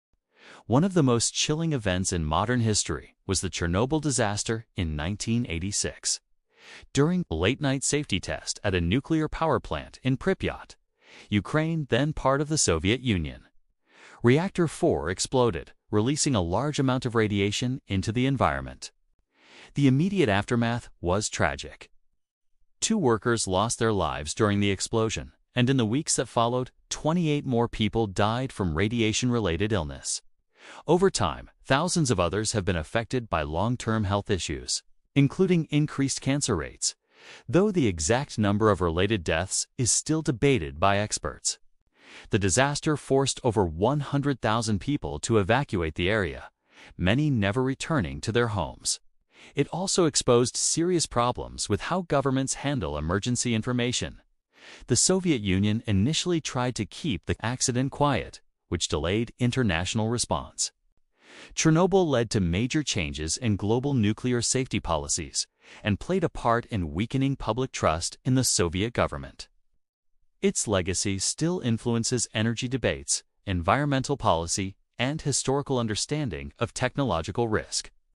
The 36-Hour Delay That Made Chernobyl Worse
The reactor explosion was bad. The Soviet Union's decision to hide it was catastrophic. Chernobyl's legacy isn't just radiation—it's a masterclass in how not to handle a disaster, changing global safety and politics forever.